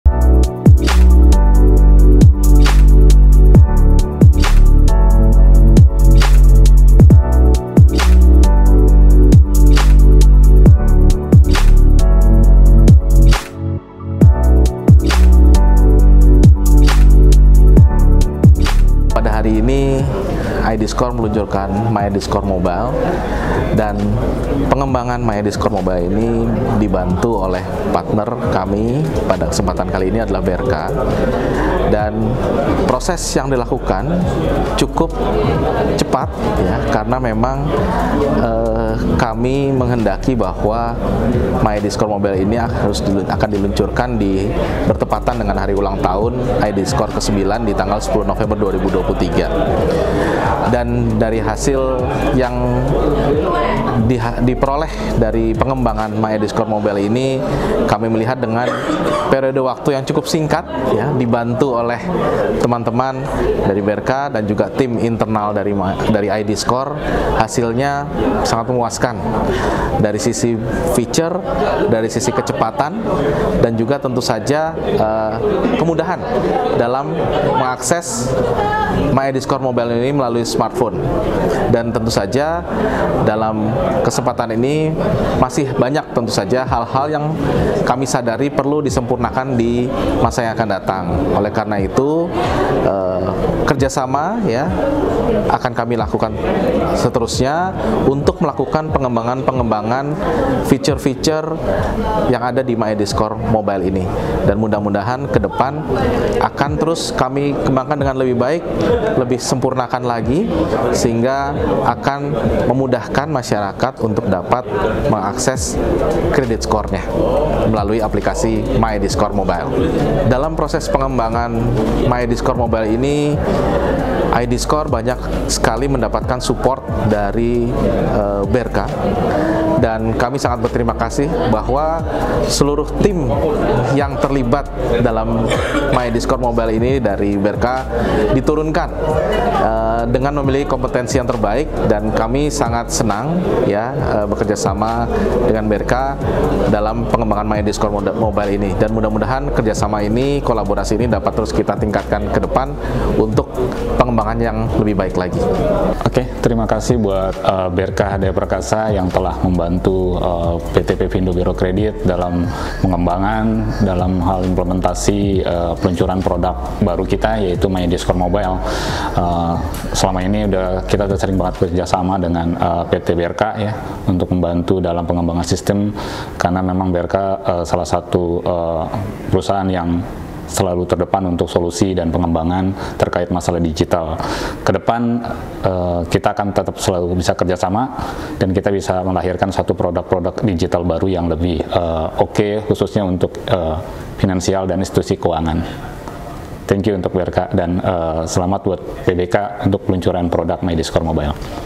Launching MyIdScore, Berca Hardayaperkasa Dibalik Kesuksesan dan Kemudahan Fitur Aplikasi
Hallo Sobat BercaOne
Pasti tidak asing dengan Credit Score bukan? Kali ini untuk memudahkan cara untuk mengecek credit score, Pefindo meluncurkan aplikasi MyidScore.

Dengan adanya kemajuan di era serba digital di mana pengajuan untuk pengecekan yang serba manual tidak lagi dibutukan serta digantikan dengan penggunaan aplikasi ini yang pasti akan memudahkan para pengguna yang ingin melakukan pengecekan credit score.

Melalui proyek ini, Berca Hardayaperkasa membuktikan dapat memberikan upaya yang optimal dan terbaik bersama dengan tim Pefindo IdScore untuk mengsukseskan proyek implementasi aplikasi ini.

Ingin tahu selengkapnya success story tentang project ini?
Nonton langsung sekarang!